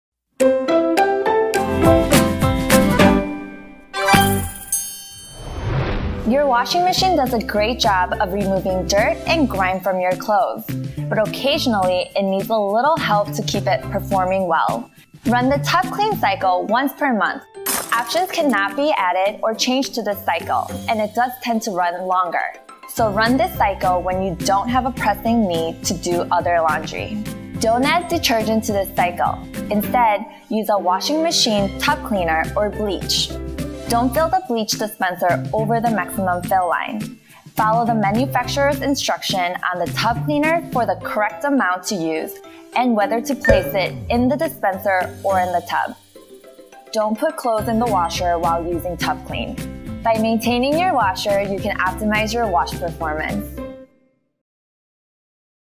LG Washer - The Tub Clean Cycle
Run the Tub Clean cycle once per month to help keep your LG washing machine performing well.